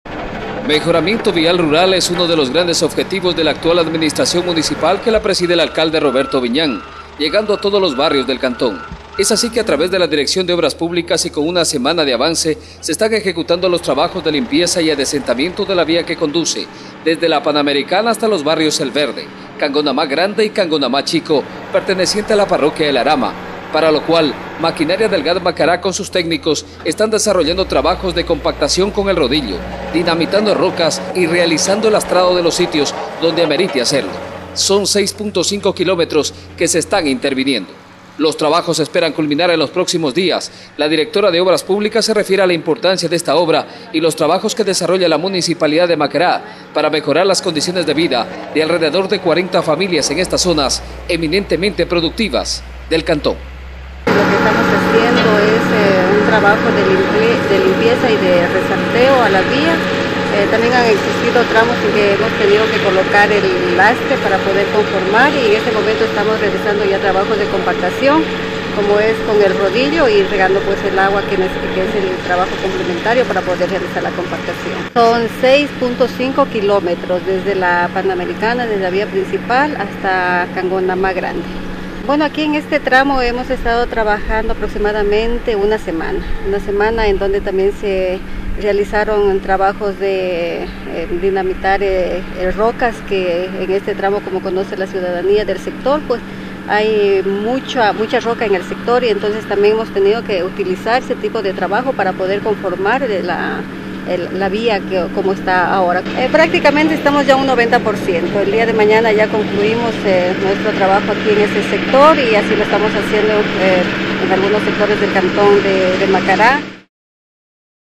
LARAMA SE BENEFICIA CON MEJOR VIALIDAD
Mejoramiento vial rural es uno de los grandes objetivos de la actual administración municipal que la preside el Alcalde, Roberto Viñán, llegando a todos los barrios del Cantón, es así que a través de la Dirección de Obras Públicas y con una semana de avance, se están ejecutando los trabajos de limpieza y adecentamiento de la vía que conduce desde la Panamericana hasta los Barrios El Verde, Cangonamá Grande y Cangonamá Chico, perteneciente a la Parroquia Larama, para lo cual maquinaria del GAD Macará con sus técnicos están desarrollando trabajos de compactación con el rodillo, dinamitando rocas y realizando el lastrado en los sitios donde amerite hacerlo, son 6.5 kilómetros que se están interviniendo.

Los trabajos se esperan culminar en los próximos días, la Directora de Obras Públicas se refiere a la importancia de esta obra y los trabajos que desarrolla la Municipalidad de Macará para mejorar las condiciones de vida de alrededor de 40 familias de estas zonas eminentemente productivas del Cantón.